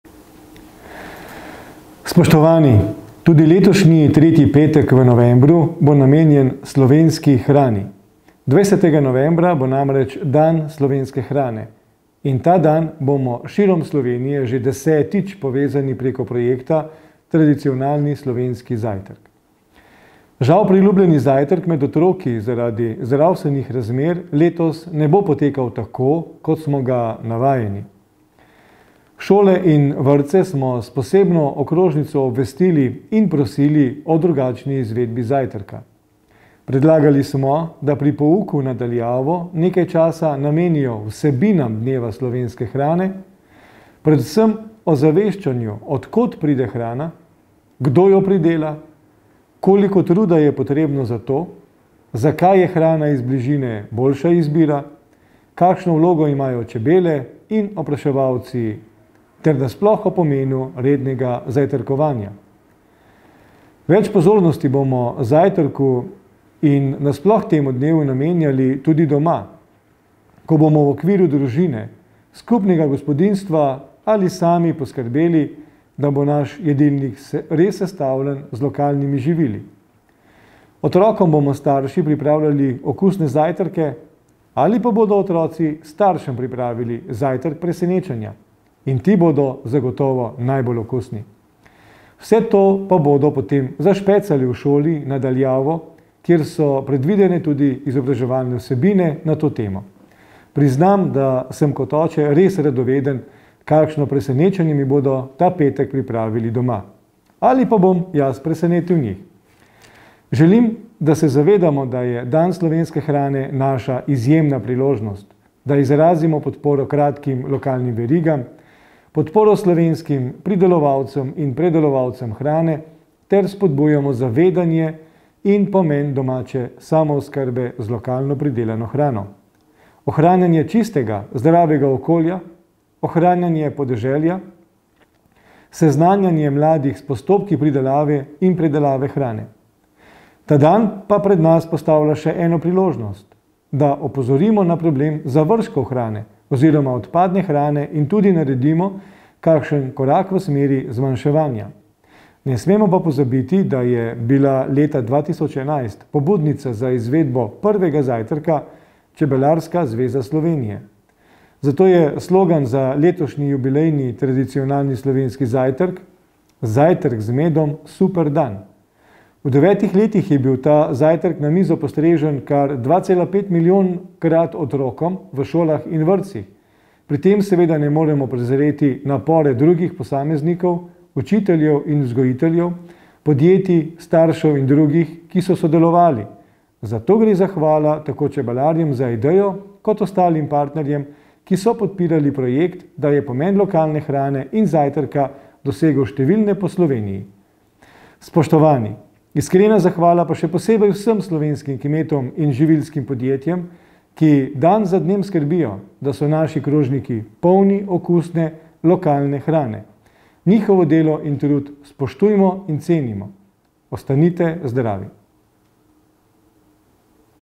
Izjava dr. Jožeta Podgorška, ministra za kmetijstvo, gozdarstvo in prehrano ob izvedbi TSZ
Minister za kmetijstvo, gozdarstvo in prehrano dr. Jože Podgoršek je ob izvedbi Tradicionalnega slovenskega zajtrka 2020 poudaril: »Tudi letošnji tretji petek v novembru bo namenjen slovenski hrani. 20. novembra bo namreč dan slovenske hrane in ta dan bomo širom Slovenije že desetič povezani preko projekta Tradicionalni slovenski zajtrk. Žal priljubljeni zajtrk pri otrokih zaradi zdravstvenih razmer letos ne bo potekal tako, kot smo ga navajeni. Šole in vrtce smo s posebno okrožnico obvestili in prosili za drugačno izvedbo zajtrka. Predlagali smo, da pri pouku na daljavo nekaj časa namenijo vsebinam dneva slovenske hrane, predvsem ozaveščanju, od kod pride hrana, kdo jo pridel, koliko truda je potrebnega za to, zakaj je hrana iz bližine boljša izbira, kakšno vlogo imajo čebele in opraševalci ter nasploh o pomenu rednega zajtrkovanja. Več pozornosti temu dnevu bomo namenili tudi doma, ko bomo v okviru družine, skupnega gospodinjstva ali sami poskrbeli, da bo naš jedilnik res sestavljen z lokalnimi živili.
Želim si, da se zavedamo, da je dan slovenske hrane naša izjemna priložnost, da izrazimo podporo kratkim lokalnim verigam, podporo slovenskim pridelovalcem in predelovalcem hrane ter spodbujamo zavedanje o pomenu domače samooskrbe z lokalno pridelano hrano.«